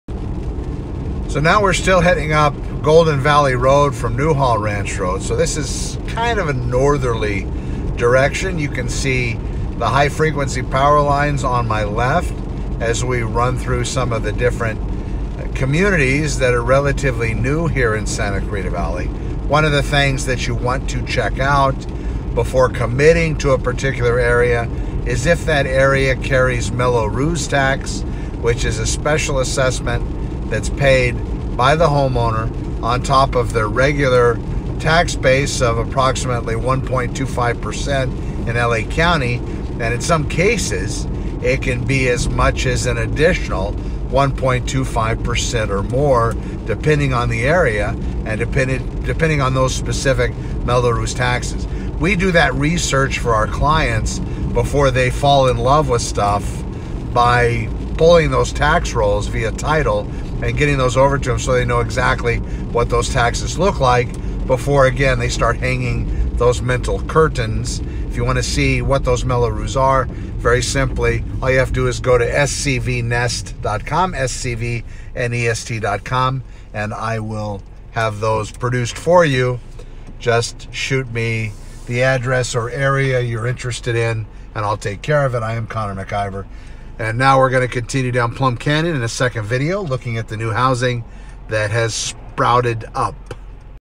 Get Mello Roos tax amount before falling in love
What about Mello Roos taxes on Santa Clarita homes and especially those new home communities. Before you fall in love let me introduce you to our online Mello Roos portal you will be able to make an inquiry and I will get you the Mello Roos tax amounts ASAP. No cost and for free. If you would have me as your real estate representative that would be super awesome. Be safe and hit me up when you are ready to have your real estate won and not just done.